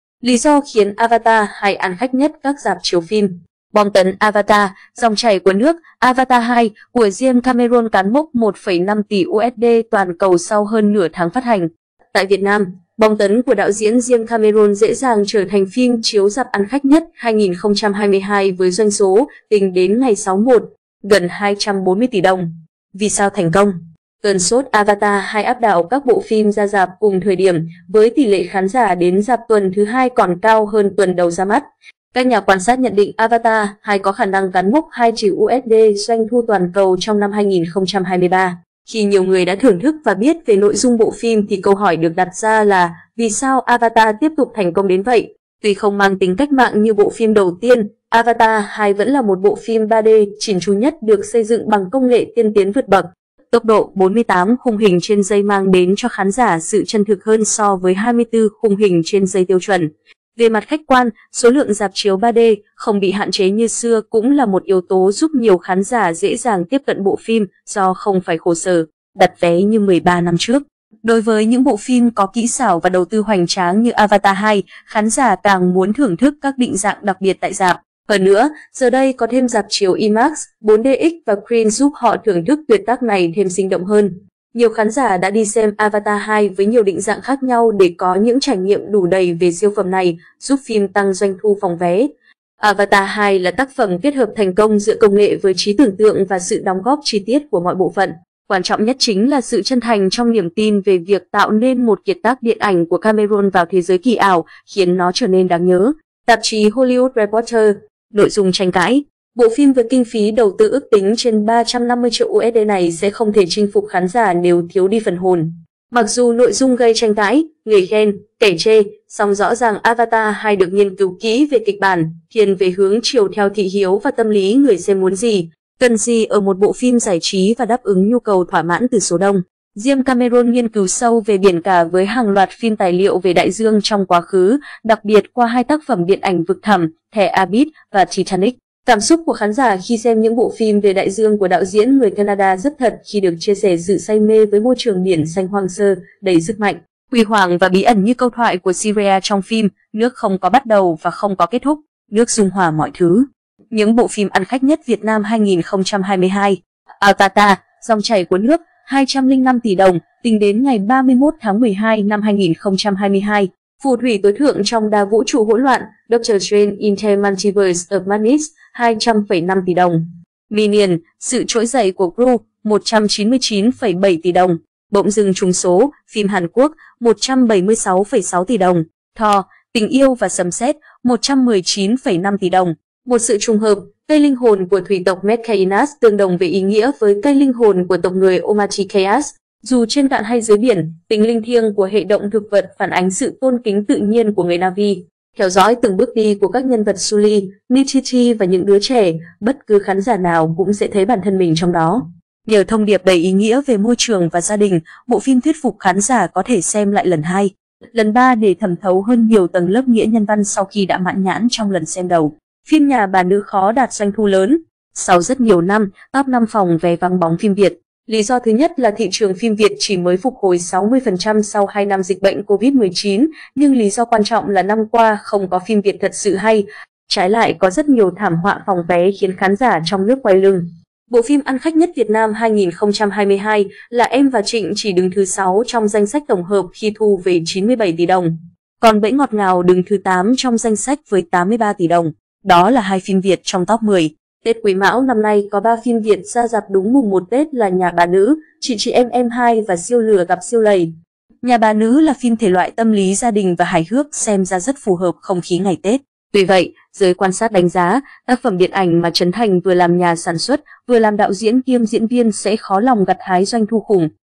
Lý do khiến 'Avatar 2' ăn khách nhất các rạp chiếu phim
Bom tấn 'Avatar: Dòng chảy của nước' (Avatar 2) của James Cameron cán mốc 1,5 tỷ USD toàn cầu sau hơn nửa tháng phát hành. Tại Việt Nam, bom tấn của đạo diễn James Cameron dễ dàng trở thành phim chiếu rạp ăn khách nhất 2022 với doanh số (tính đến ngày 6-1) gần 240 tỷ đồng.James Cameron nghiên cứu sâu về biển cả với hàng loạt phim tài liệu về đại dương trong quá khứ, đặc biệt qua 2 tác phẩm điện ảnh “Vực thẳm” (The Abyss) và “Titanic”. Cảm xúc của khán giả khi xem những bộ phim về đại dương của đạo diễn người Canada rất thật khi được chia sẻ sự say mê với môi trường biển xanh hoang sơ đầy sức mạnh, huy hoàng và bí ẩn như câu thoại của Tsireya trong phim: “Nước không có bắt đầu và không có kết thúc. Nước dung hòa mọi thứ”.
Bộ phim với kinh phí đầu tư ước tính trên 350 triệu USD này sẽ không thể chinh phục khán giả nếu thiếu đi “phần hồn”. Mặc dù nội dung gây tranh cãi (người khen, kẻ chê), song rõ ràng “Avatar 2” được nghiên cứu kỹ về kịch bản, thiên về hướng chiều theo thị hiếu và tâm lý người xem muốn gì, cần gì ở một bộ phim giải trí và đáp ứng nhu cầu thỏa mãn từ số đông.
Tạp chí Hollywood Reporter
“Avatar: Dòng chảy của nước” là sự kiện điện ảnh lớn nhất cuối năm 2022, đầu năm 2023
Khi nhiều người đã thưởng thức và biết về nội dung bộ phim thì câu hỏi được đặt ra là: “Vì sao “Avatar” tiếp tục thành công đến vậy?”. Tuy không mang tính cách mạng như bộ phim đầu tiên, “Avatar 2” vẫn là một bộ phim 3D chỉn chu nhất được xây dựng bằng công nghệ tiên tiến vượt bậc. Tốc độ 48 khung hình/giây mang đến cho khán giả sự chân thực hơn so với 24 khung hình/giây tiêu chuẩn. Về mặt khách quan, số lượng rạp chiếu 3D không bị hạn chế như xưa cũng là một yếu tố giúp nhiều khán giả dễ dàng tiếp cận bộ phim (do không phải “khổ sở” đặt vé như 13 năm trước). Đối với những bộ phim có kỹ xảo và đầu tư hoành tráng như “Avatar 2”, khán giả càng muốn thưởng thức các định dạng đặc biệt tại rạp. Hơn nữa, giờ đây có thêm rạp chiếu IMAX, 4DX và ScreenX giúp họ thưởng thức tuyệt tác này thêm sinh động hơn. Nhiều khán giả đã đi xem “Avatar 2” với nhiều định dạng khác nhau để có những trải nghiệm đủ đầy về siêu phẩm này, giúp phim tăng doanh thu phòng vé.
Nội dung tranh cãi
- “Phù thủy tối thượng trong đa vũ trụ hỗn loạn” (Doctor Strange in the Multiverse of Madness): 200,5 tỷ đồng
- “Bỗng dưng trúng số” (phim Hàn Quốc): 176,6 tỷ đồng
Cơn sốt “Avatar 2” áp đảo các bộ phim ra rạp cùng thời điểm, với tỉ lệ khán giả đến rạp tuần thứ 2 còn cao hơn tuần đầu ra mắt. Các nhà quan sát nhận định “Avatar 2” có khả năng cán mốc 2 tỷ USD doanh thu toàn cầu trong năm 2023.
- “Thor: Tình yêu và sấm sét”: 119,5 tỷ đồng
- “Minions: Sự trỗi dậy của Gru”: 199,7 tỷ đồng
- “Avatar: Dòng chảy của nước”: 205 tỷ đồng (tính đến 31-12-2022)
“Avatar 2” là tác phẩm kết hợp thành công giữa công nghệ với trí tưởng tượng và sự đóng góp chi tiết của mọi bộ phận. Quan trọng nhất chính là sự chân thành trong niềm tin về việc tạo nên một kiệt tác điện ảnh của Cameron vào thế giới kỳ ảo khiến nó trở nên đáng nhớ.
Những bộ phim ăn khách nhất Việt Nam 2022
Vì sao thành công?Một sự trùng hợp, cây linh hồn của thủy tộc Metkayinas tương đồng về ý nghĩa với cây linh hồn của tộc người Omatikayas. Dù trên cạn hay dưới biển, tính linh thiêng